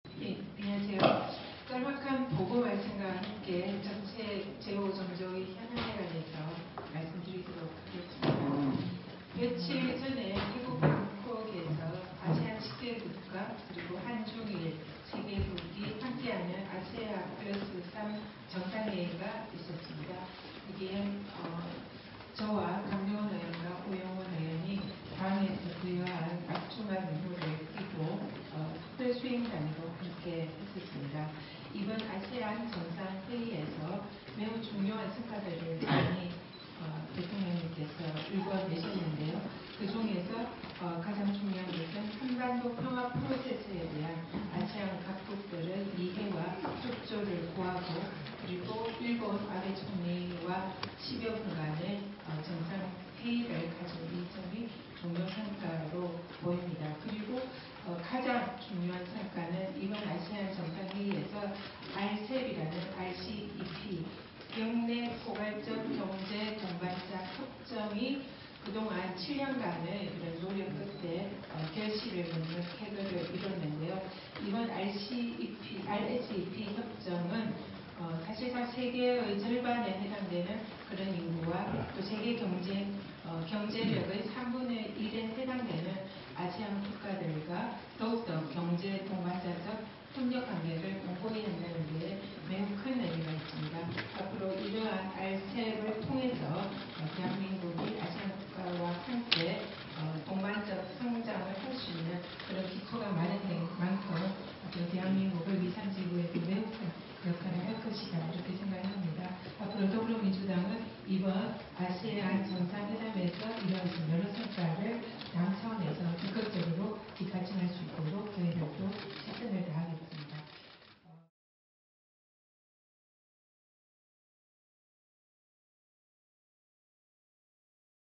문재인 대통령, 아세안+3 정상회의에서 중요한 성과를 일구어냈습니다
전현희 의원 "문재인 대통령, 아세안+3 정상회의에서 중요한 성과를 일구어냈습니다"

- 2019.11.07 더불어민주당 정책조정회의 공개 발언

★문재인 대통령 아세안+3 정상회의 성과★

① 역내포괄적경제동반자협정(RCEP) 타결

- 역내포괄적경제동반자협정(RCEP) 정상회의에서 협상 7년만에 협정문 타결을 선언했습니다. 아시아태평양 지역의 ‘메가FTA’로 불리는 RCEP 타결로 세계 인구의 절반, 세계 총생산의 3분의 1을 차지하는 세계 최대 규모의 자유무역이 시작될 예정입니다^^

② 아베 일본 총리와의 11분간의 만남

- 문재인 대통령과 아베 총리는 한일관계가 중요하다는 데 의견을 같이하며 한일 양국 관계의 현안은 대화를 통해 해결해야 한다는 원칙을 재확인했습니다.

③ ‘한-아세안 특별정상회의’ 및 ‘한-메콩 정상회의’의 성공적 개최를 위한 발판 마련

- 문재인 대통령은 11월 25일부터 부산에서 개최되는 한·아세안 특별정상회의와 한·메콩 정상회의의 성공적 개최를 위한 아세안 국가 정상의 협력을 당부했습니다.